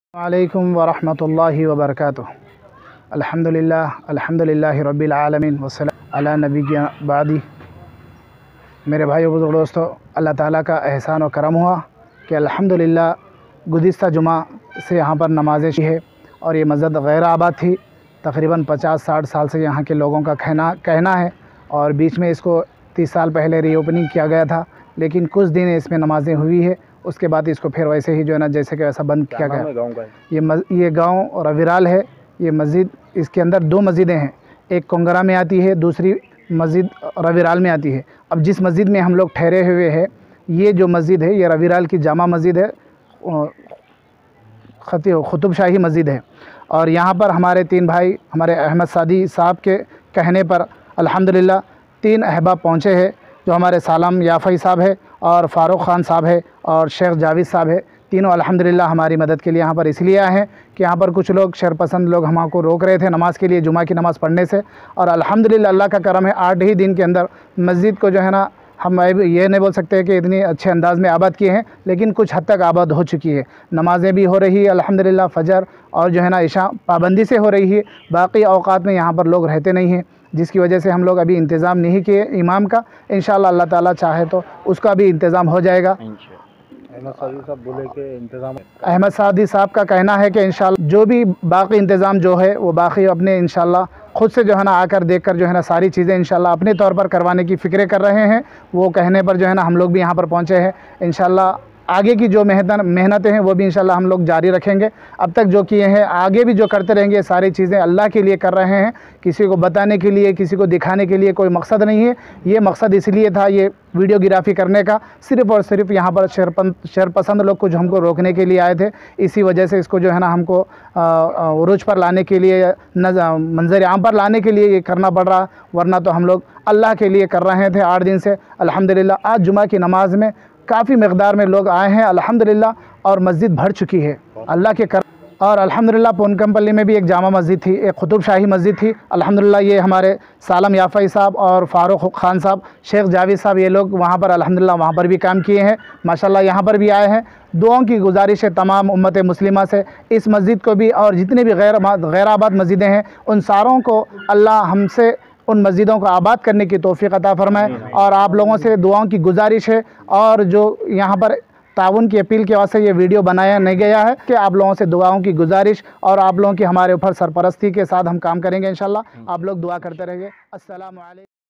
NIZAM KI EK GAIR ABAD MASJID KO AAJ ABAD KIYA GAYA
RAIVIRAL VILLAGE MEIN DOURE NIZAM KI EK QAIR ABAD MASJID KO AJJ ABAD KIYA GAYA ALHAMDULILLAH..AJJ JUME KI NAMAZ PADNE KE LIYE PAHADI SHAREEF AUR SHAHEEN NAGAR KI AWAAM WAHA NAMAZ PADNE KE LIYE JAMA HOWE THE..KUCH SHARPASAND ANASIR LOGO NE NAMAZ PADNE SE ROUK NE KI KOUSHIS KI. PATA CHALTE HI JALPALLY INCHARGE AIMIM AHMED SADI SAHAB NE...SALAM YAFAI AUR FAROOQ KHAN AUR SHIAK JAWEED KO WAHA BEHJ KAR NA SIRF UN SHARPASAND HATAYA BALKE WAHA JUMMA KI NAMAZ BHI AADA KI IS MOUQE PAR WAHA LOGO NE AHMED SADI SAHAB AUR ..SALAM YAFAI
FAROOQ KHAN SHAIK JAWEED KA SHUKRIYA ADDA KIYA. .
NAZRIN APKO PATA HOGA YEH 3 WOHI REAL HERO JINHO NE KUCH DIN PAHLE IBRAHIM PATNAM MANDAL MEIN EK QAIR AABAD QUTUB SHAHI MASJID KO ABAD KIYA THA. AJJ PHIR SE YEH...AHMED SADI SAHAB KE KEHNE PAR RAIVIRAL VILLAGE MEIN BHI EK AUR MASJID KO AABAD KIYA